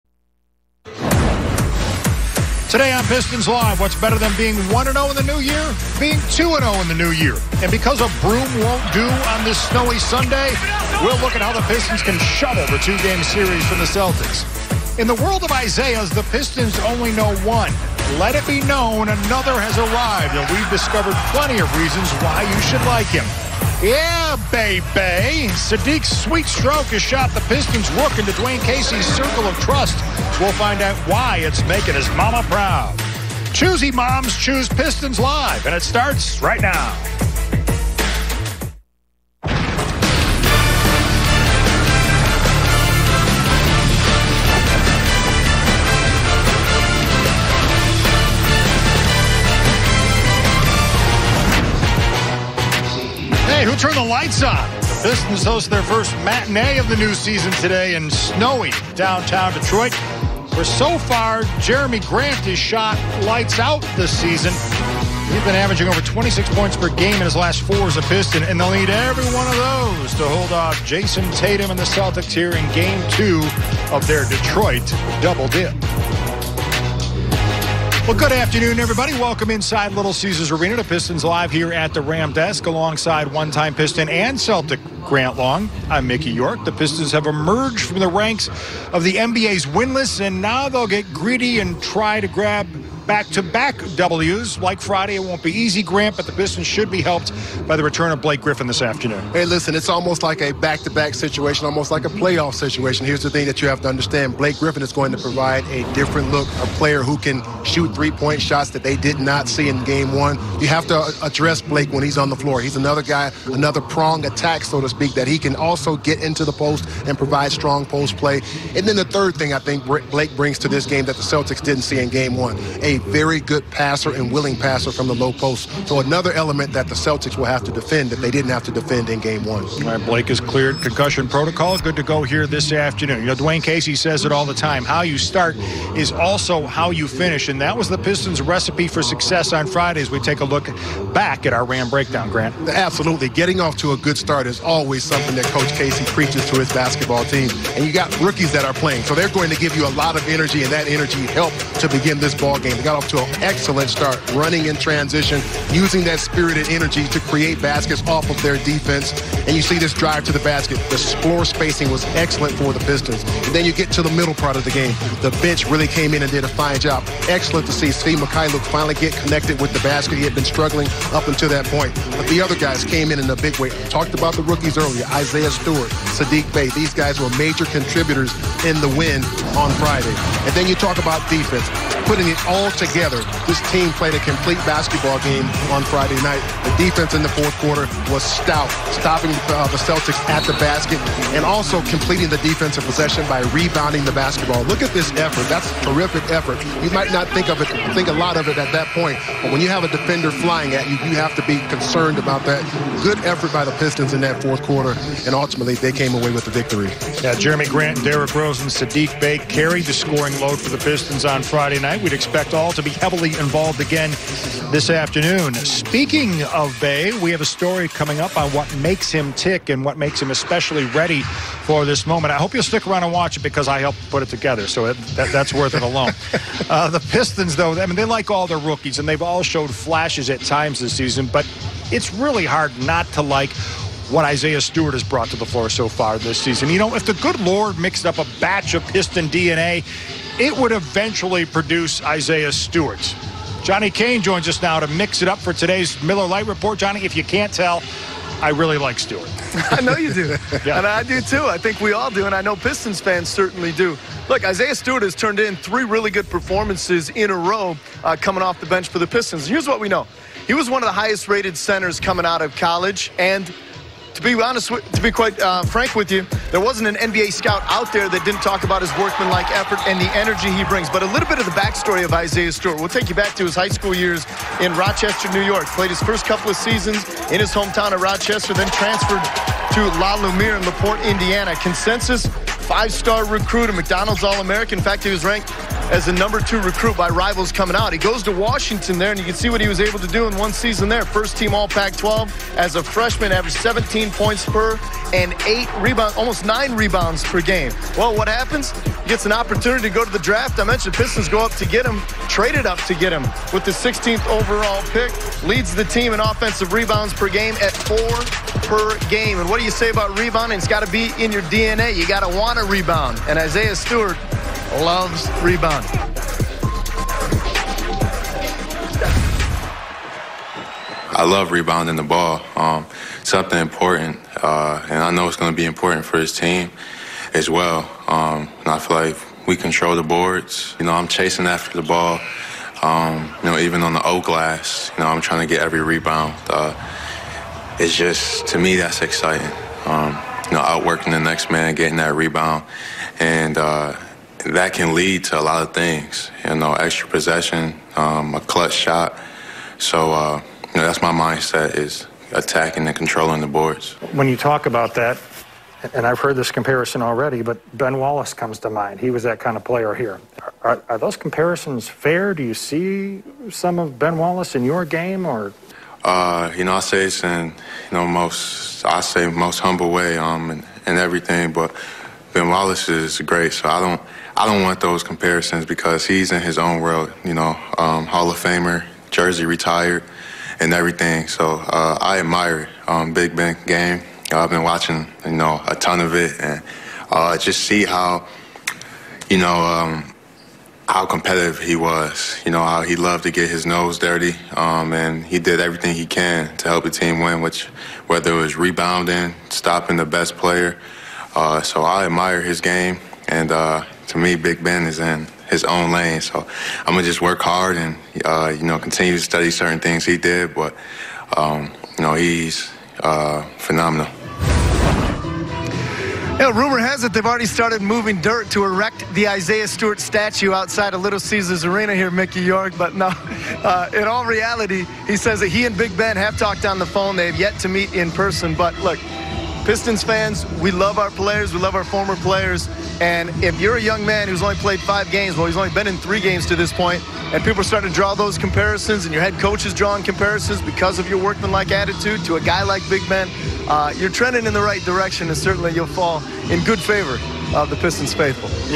Let's Start A Streak
Pistons LIVE is priming you for today's matchup between the Celtics and your Detroit Pistons. We're looking at what it took to get win #1 on Friday, plus Johnny Kane gives us a look at Isaiah Stewart.